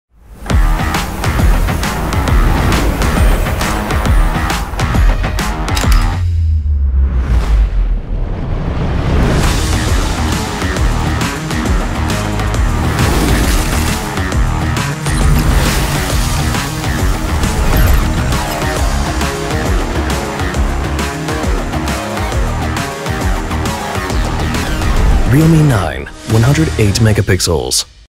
realme 9 | Now Available across Pakistan
Capture All-Day Clarity with realme 9.

A Mid-range stunner; realme9 is what we all desire, equipped with the most advanced specs;
☆108MP Ultra Clear Camera
☆First-ever HM6 Sensor
☆Ripple Holographic Design
☆Snapdragon 680 6nm Processor
☆90Hz sAMOLED Display
☆5000mAh Massive Battery and much more.

Now Available across Pakistan for Rs.54,999. Grab Yours Today!